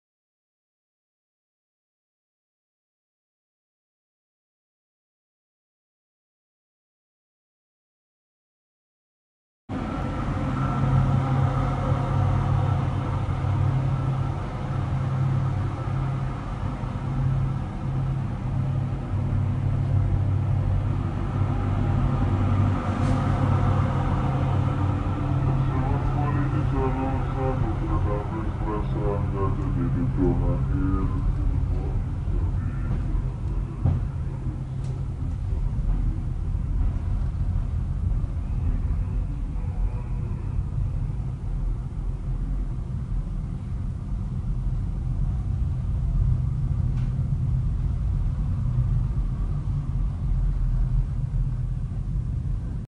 Kereta Luar Biasa Lokomotif CC 300 12 02
Kereta Api buatan INKA Indonesia